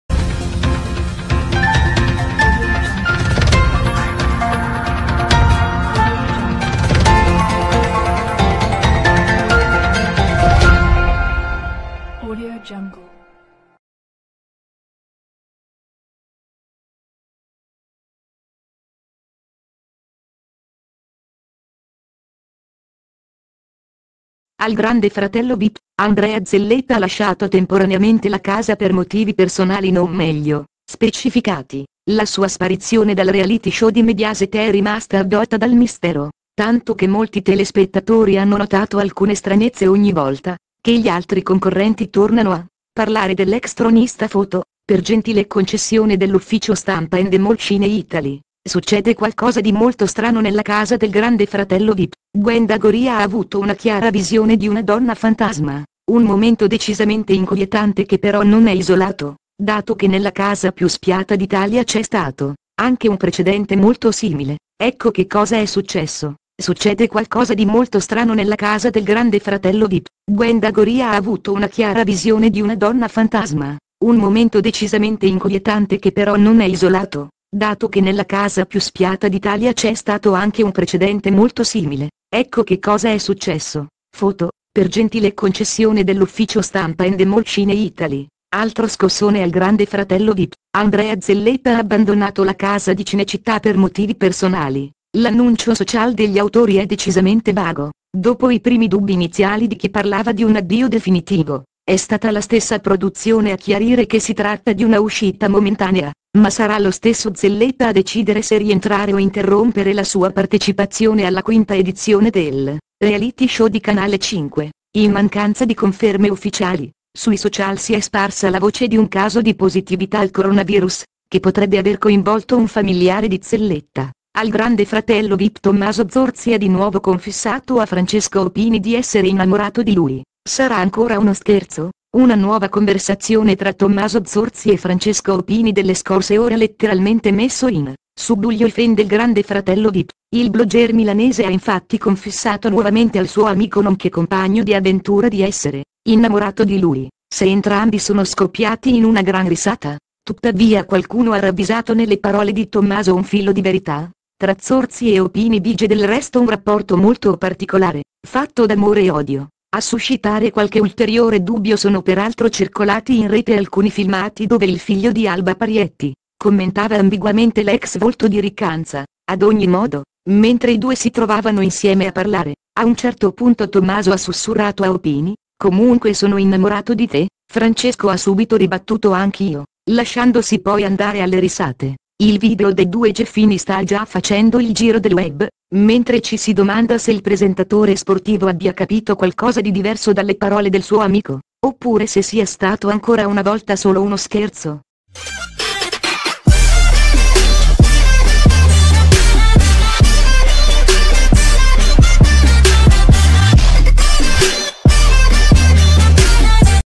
Andrea Zelletta esce dal GF Vip per "motivi personali": la sparizione è avvolta dal mistero
ANDREA ZELLETTA